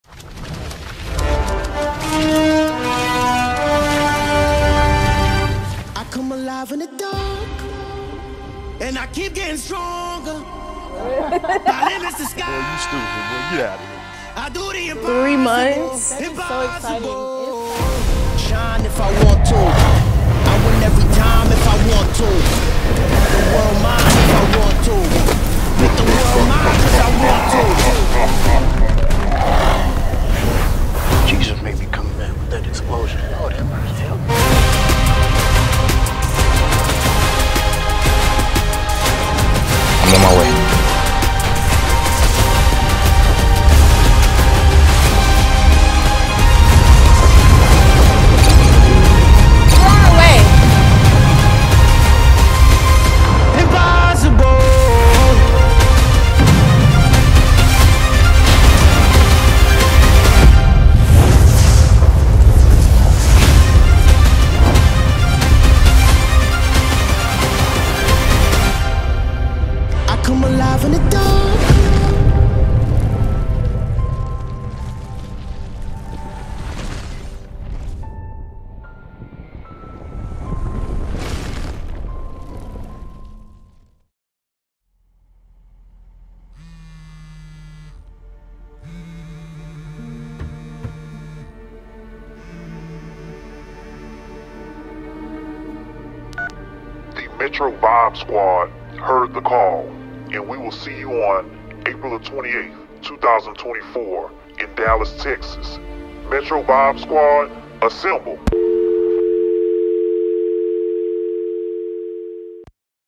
2024 Revive the Culture HBCU Power Camp: Enter the Art-Zverse Movie Trailer Promo
🌟✨ Breaking News in the Art-Zverse! 🎬🚀 Prepare for an epic journey as SEVEN Art-Zverse Superheroes unite to save our realm! The stakes have never been higher, and the call for heroes has been answered. 🦸‍♀️🦸‍♂️
🎥 The moment you’ve all been waiting for: The official movie trailer drops on February 14, 2024! Mark your calendars for a Valentine’s Day filled with thrill, adventure, and art like you’ve never seen before. 💖🌌
Get ready to witness the power of creativity and courage combined. The Art-Zverse needs its heroes, and they’re arriving just in time. 🌍✊